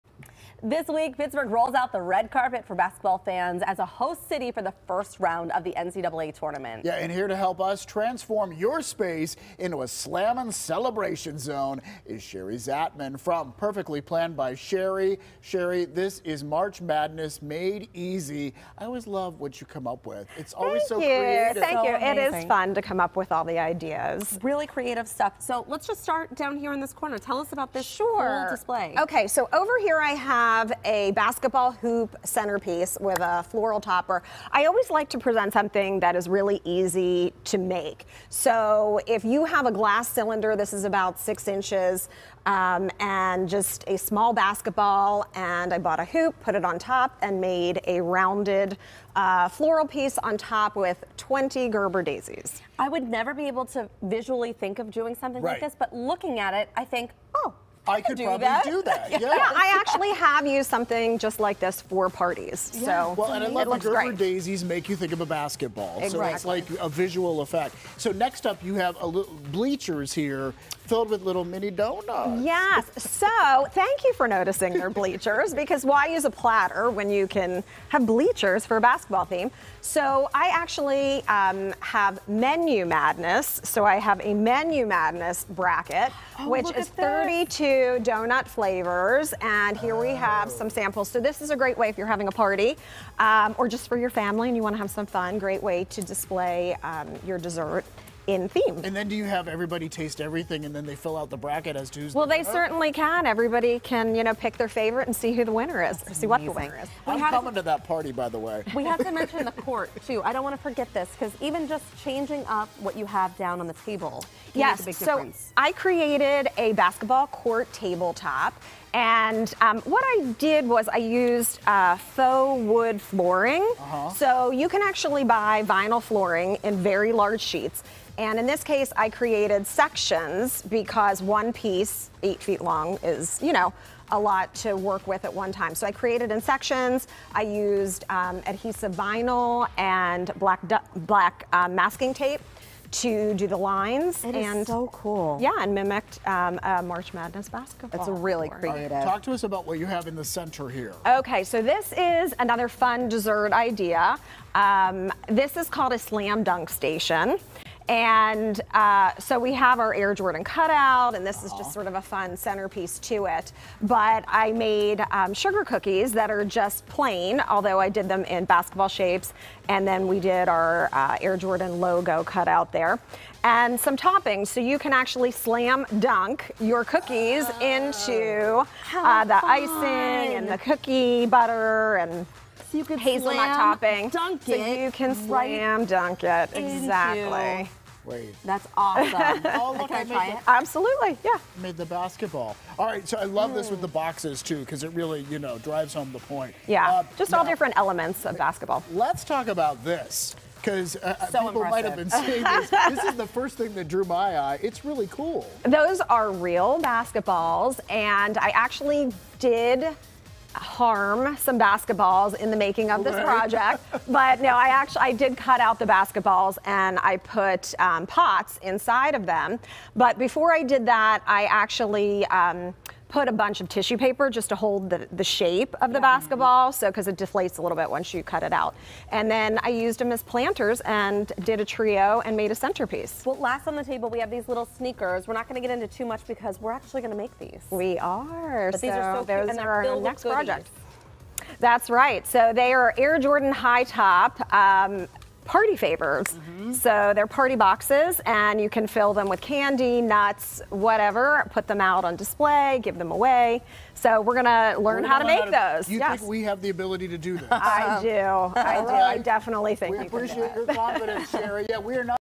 Decking out the hoops party of your dreams with Shari Zatman
Pittsburgh is rolling out the red carpet for basketball fans as a host city for the first round of the NCAA Tournament. Shari Zatman was here to help us transform your space into a slammin' celebration zone.